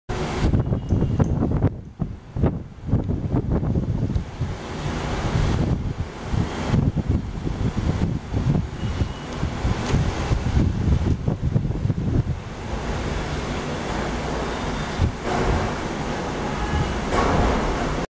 Construction Crane / Crain
Construction Crane / Crain at Mumbai. At the vicinity of cyclone Tauktae this video is recorded.